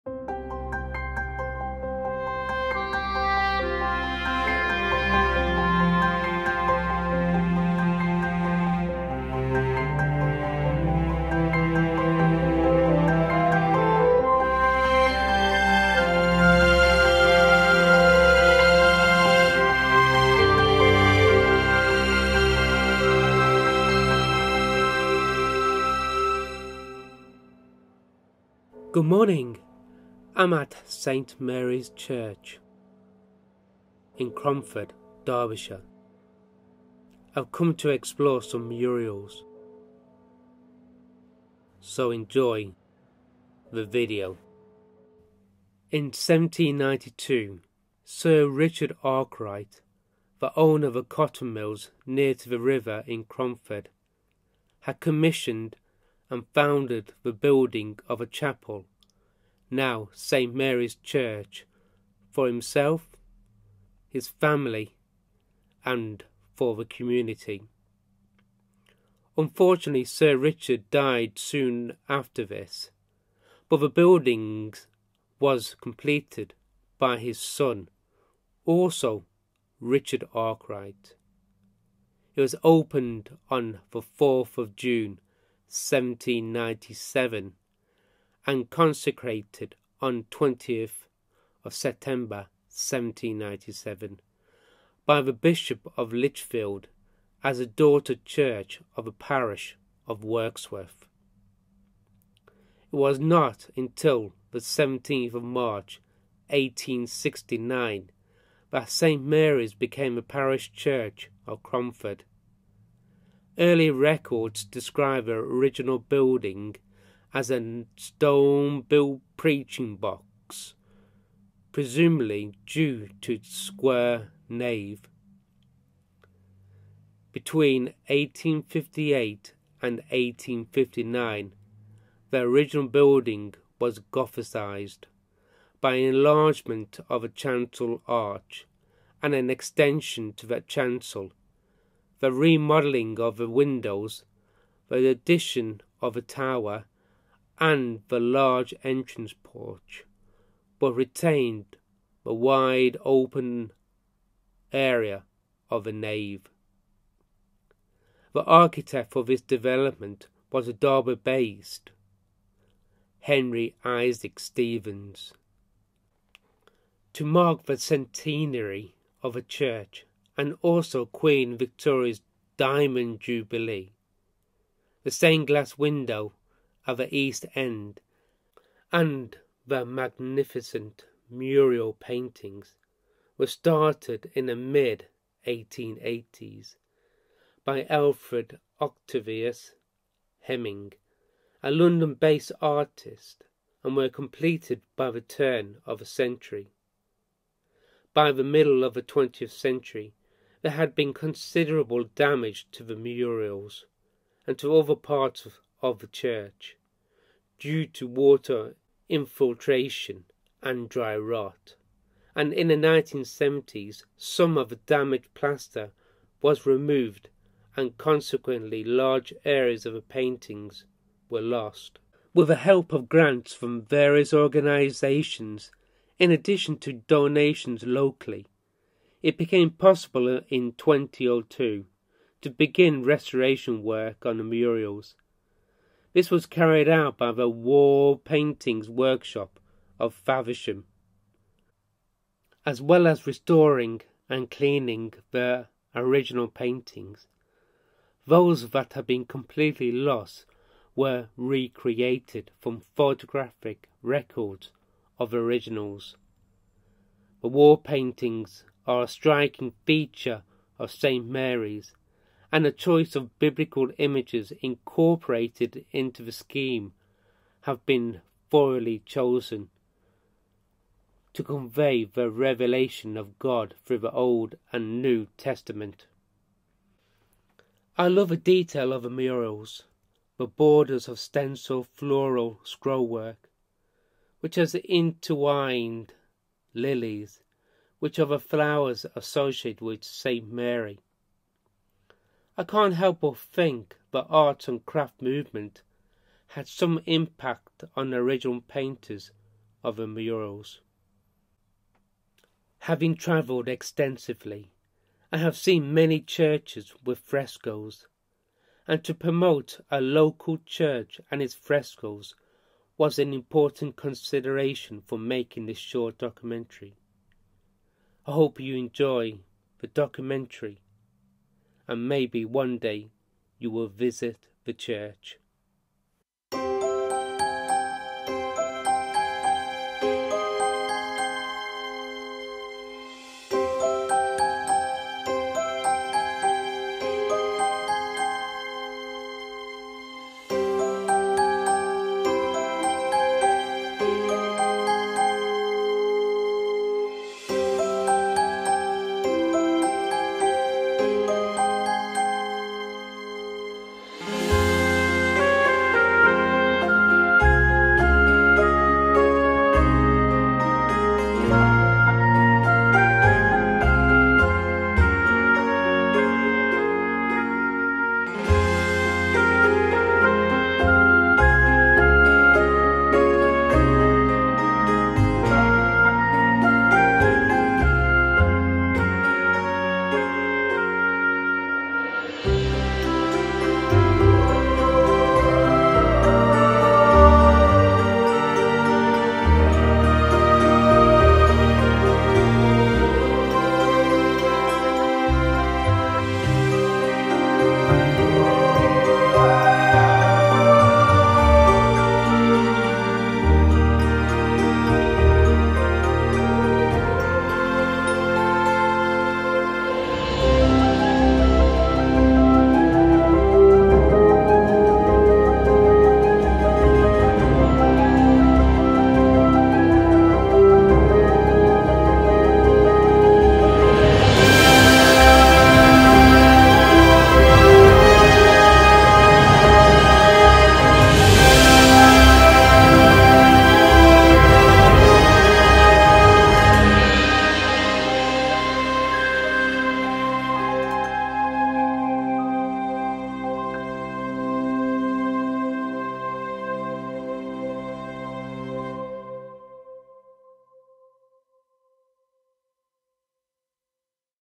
Admiring St Mary's Church and its Murals or Frescos in Cromford, Derbyshire
In 1792, Sir Richard Arkwright, the owner of the cotton mills near the river in Cromford, had commissioned and founded the building of a chapel, now St Mary’s Church, for himself, his family and the community.

Unfortunately, Sir Richard died soon after this, but the building was completed by his son, Richard Arkwright. It was opened on 4th June 1797 and consecrated on 20th September 1797 by the Bishop of Lichfield as a daughter church of the parish of Wirksworth.

It was not until 17th March 1869 that St Mary's became the parish church of Cromford. Early records describe the original building as "a stone-built preaching box" presumably due to its very square nave.

Between 1858 and 1859, the original building was ‘Gothicised’ by an enlargement of the chancel arch and an extension to the chancel, the remodelling of the windows, the addition of a tower, and the large entrance porch, but retaining the wide open area of the nave.

The architect for this development was the Derby-based Henry Isaac Stevens.
To mark the Centenary of the church, and also Queen Victoria's Diamond Jubilee, the stained glass window at the east end and the magnificent mural paintings were started in the mid-1880s by Alfred Octavius Hemming, a London-based artist and were completed by the turn of the century.

By the middle of the 20th century, there had been considerable damage to these murals and to other parts of the building due to water infiltration and dry rot, and in the 1970s, some of the damaged plaster was removed and consequently, large areas of the paintings were lost.

With the help of grants from various organisations in addition to donations locally, it became possible in 2002 to begin restoration work on the murals; this was carried out by "The Wall Paintings Workshop" of Faversham.

As well as restoring and cleaning the original frescos, those that had been completely lost were recreated from photographic records of the originals.

The wall paintings are a striking feature of St Mary’s, and the choice of biblical images incorporated in the scheme have been thoughtfully chosen to convey the revelation of God through the Old and New Testament.

I love the detail of the murals, the borders of stencilled floral scrollwork, which has intertwined lilies, which are flowers associated with St Mary. I cant help but think the arts and craft movement had some impact on the original painters of the murals.

Having travelled extensively, I have seen many churches with frescoes, and promoting a local church and its frescoes was an important consideration for making this short documentary. Hope you enjoy it and maybe one day you will visit the church.